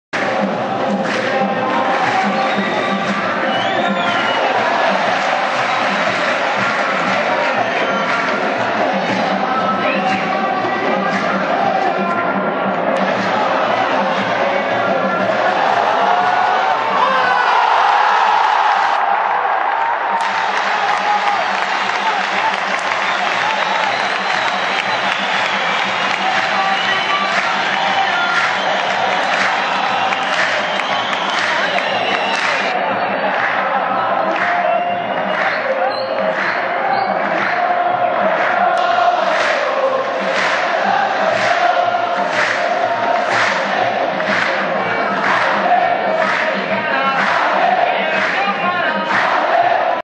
هدف الثعلب مصطفى بصاص في الوحدة تصوير ميلانيستا
الاهلي 3 _ 1 الوحدة
عدسة ميلآنيستآ